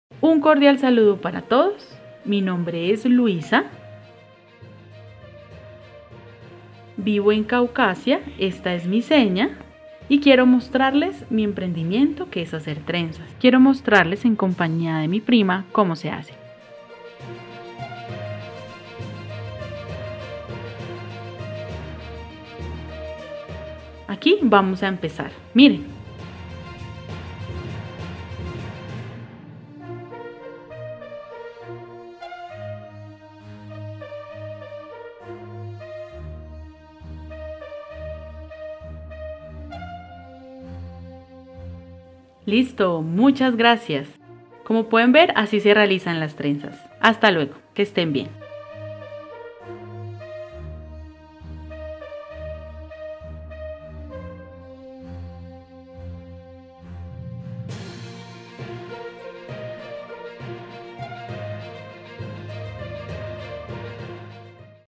Emprendedora: Luisa Gómez
Desde Caucasia (Antioquia), Luisa Gómez, nos comparte su proyecto de vida.